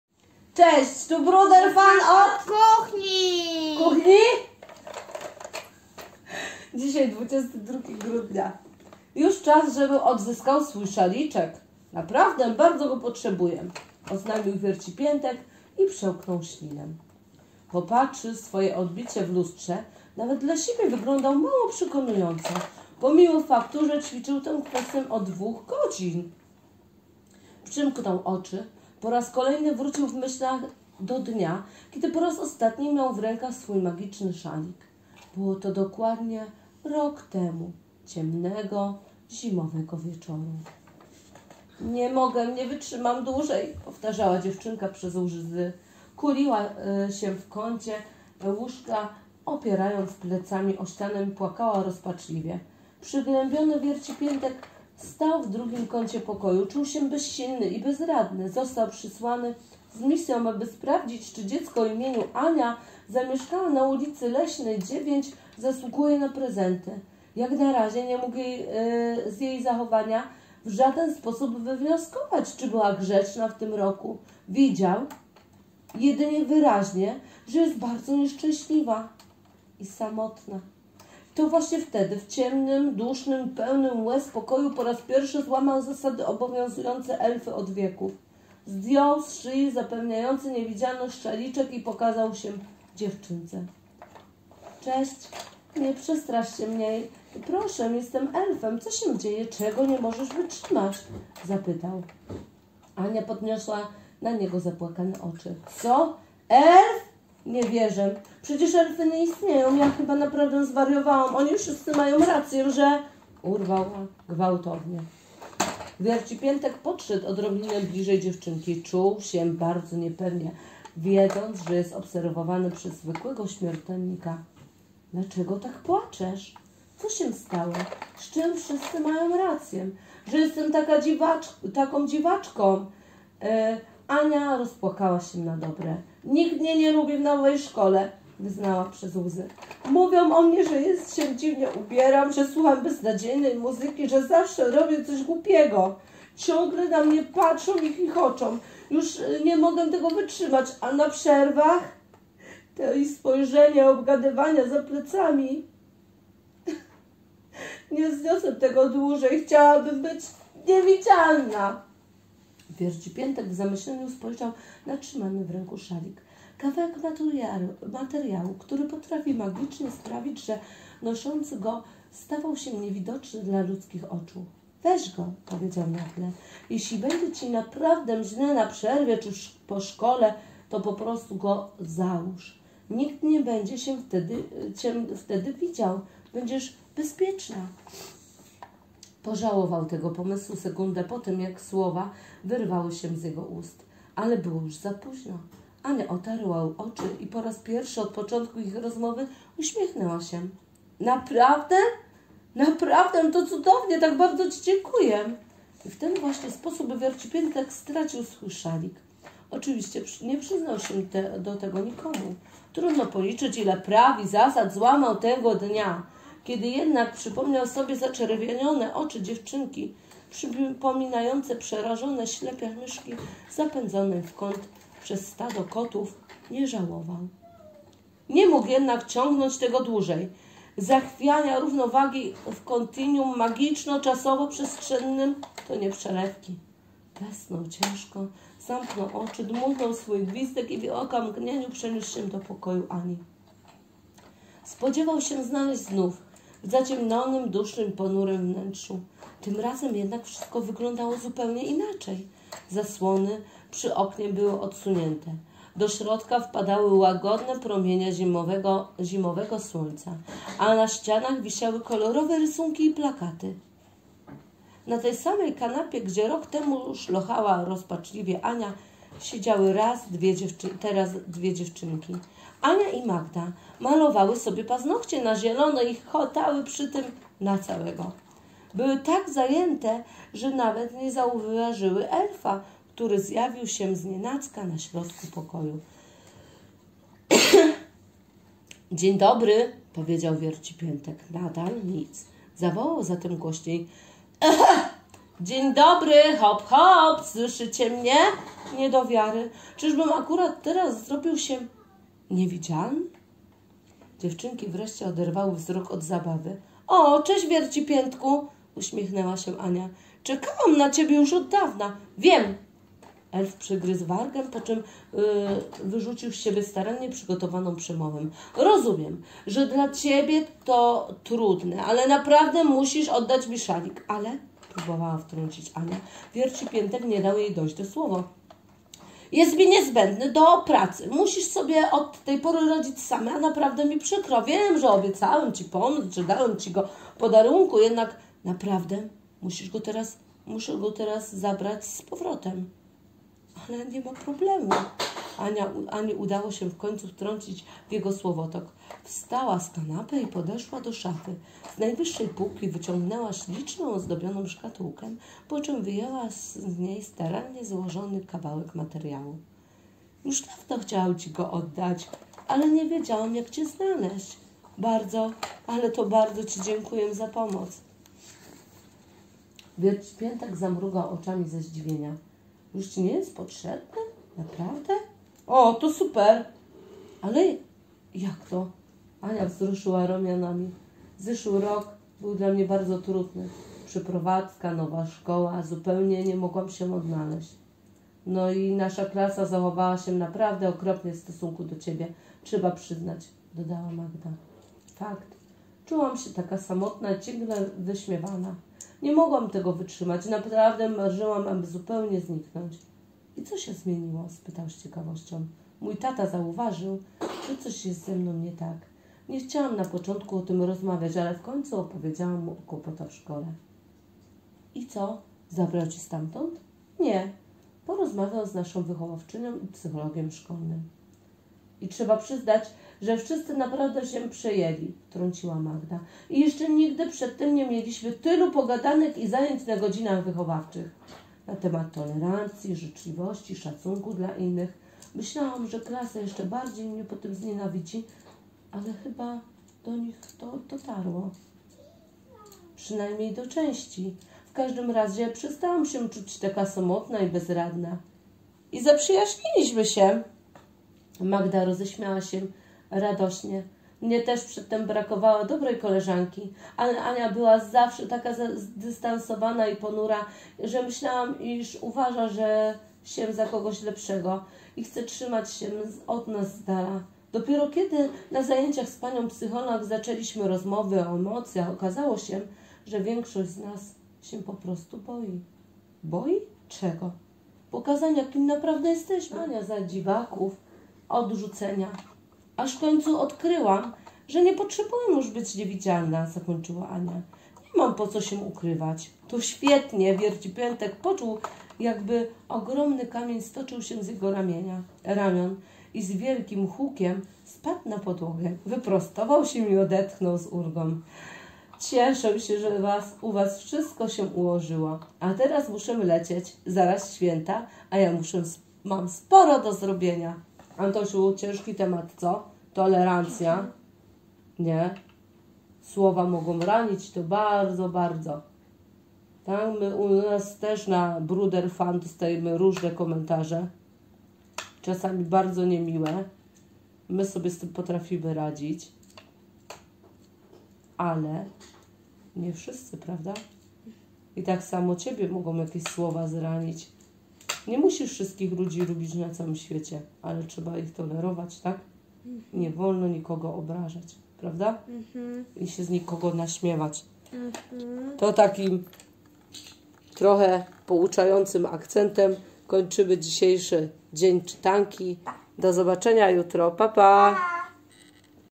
Świąteczna czytanka "Elf do zadań specjalnych" - 22 grudnia TOLERANCJA
Czytanie z Bruderami, dzisiaj Claas i Fendt. Ostatnio towarzyszą nam zabawki z Kinder Niespodzianki.
No ważny, a nawet bardzo ważny temat, który dzisiaj poruszyliśmy - tolerancja. Pamiętajcie, że nikogo nie można obrażać, poniżać i naśmiewać się z niego. Słowa często bardzo ranią i towarzyszą nam przez długi czas. Nie dajcie się nikomu obrażać i proście o pomoc rodziców czy nauczycieli albo innych bliskich. Macie prawo być szczęśliwi. Tego właśnie Wam życzymy❤
Reading with Bruders, today Claas and Fendt. Recently we have been accompanied by toys from Kinder Niespodzianka.
Well, an important, and even very important topic that we have discussed today - tolerance. Remember that no one can be offended, humiliated or made fun of. Words often hurt a lot and stay with us for a long time. Do not be offended by anyone and ask your parents, teachers or other relatives for help. You have the right to be happy. This is what we wish you to do ❤